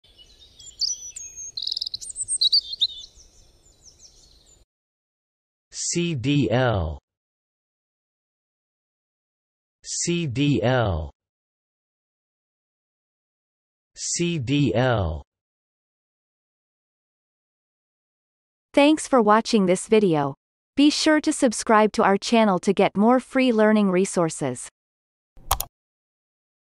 How to pronounce cdl - Vocab Today
The pronunciation of the word cdl in Amercian accent is demonstrated in this video. If the word is from another language, such as brand name, it will be pronounced in its original language. Based on the word frequency, this is of 4 stars difficulty.